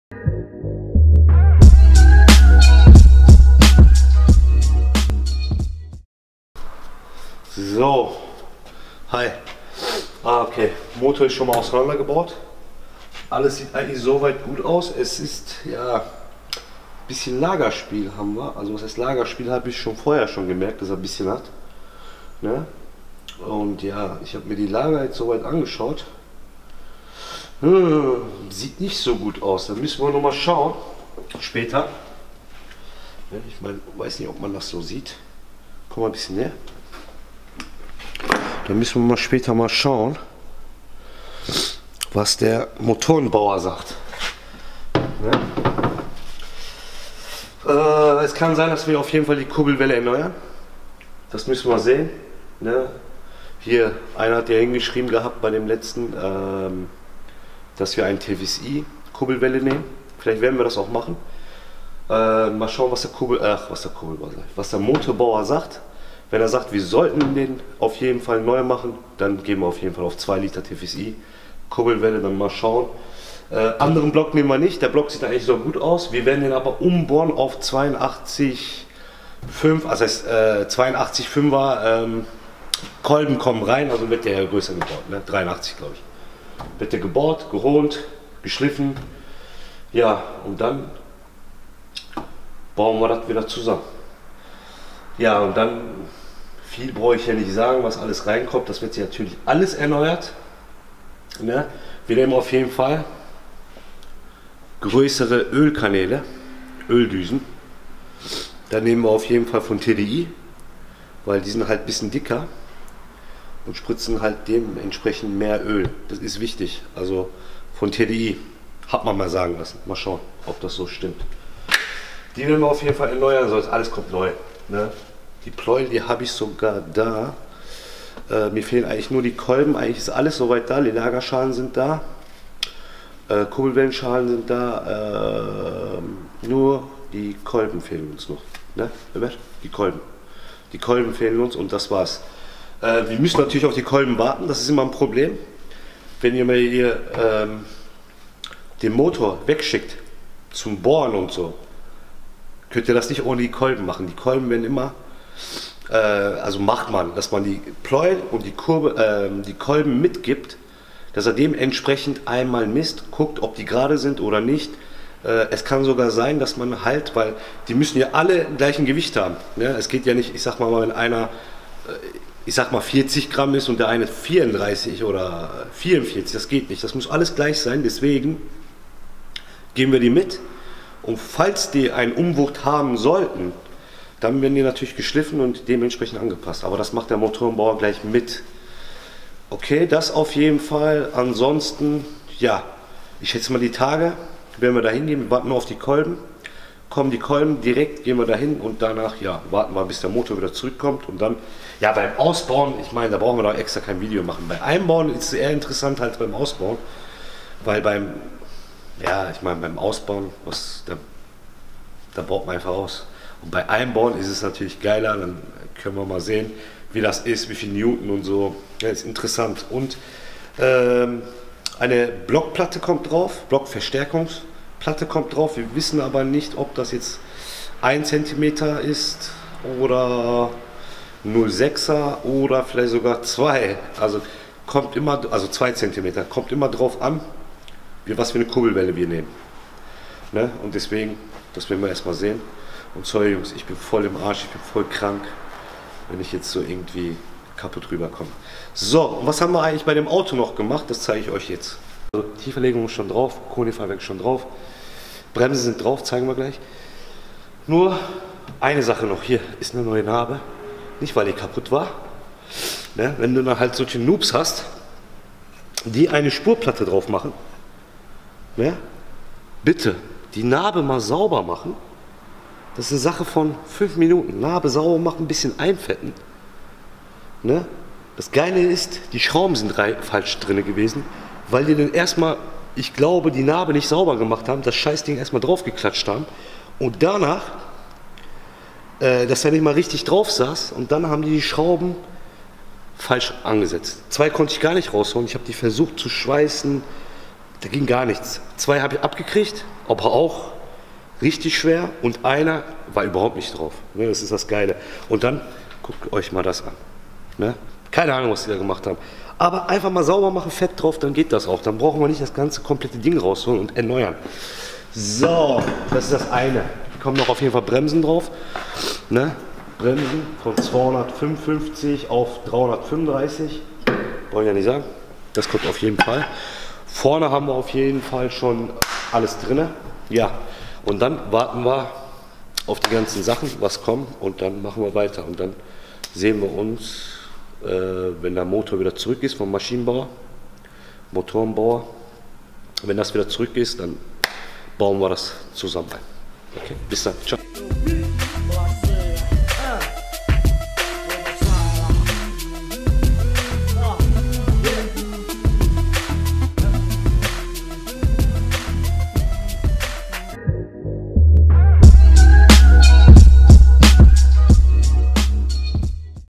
Audi 1.8t S3 8L Umbau auf 700 PS
Hier geht es nun weiter mit dem Umbau unseres Audi S3...
Motor zerlegt und Lagerspiel festgestellt, nun schauen wir, ob wir eine 2 Liter TSFI Kurbelwelle nehmen oder nicht..

Projekt: Wir bauen ein S3-Motor 1.8t um und wollen 700 PS daraus machen... wir sind sehr gespannt auf das Endergebnis und werden euch in den nächsten Videos mehr berichten... wenn Ihr uns abonniert, würden wir uns freuen und wenn Ihr Tipps habt, dann lasst es uns wissen...